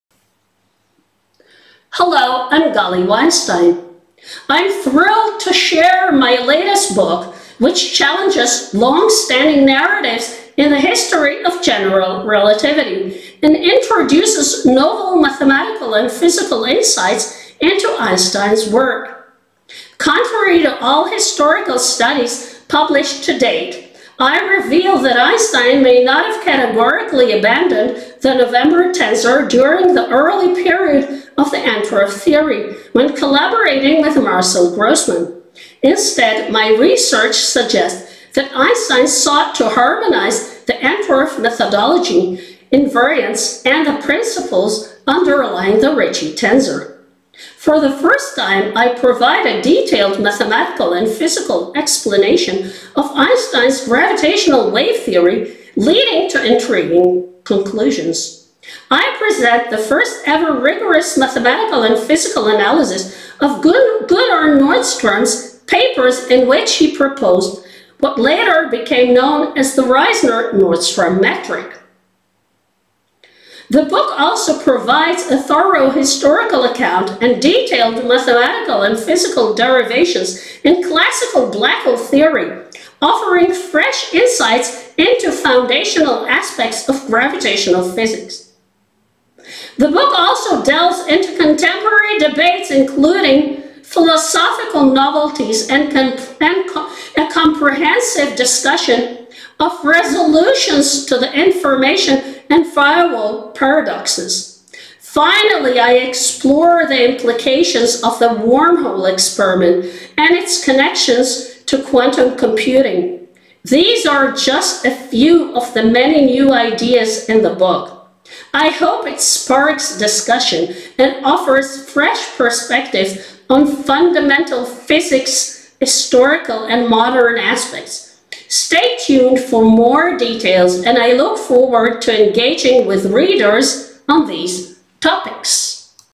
Einstein, Black Holes & Wormholes – My New Book
This video briefly introduces my new book, "Einstein's Legacy: From General Relativity to Black Hole Mysteries," which explores the evolution of general relativity and black hole research. It provides new insights into the history and development of these theories. It is intended for researchers, academics, and students in the history and philosophy of physics as well as theoretical physics.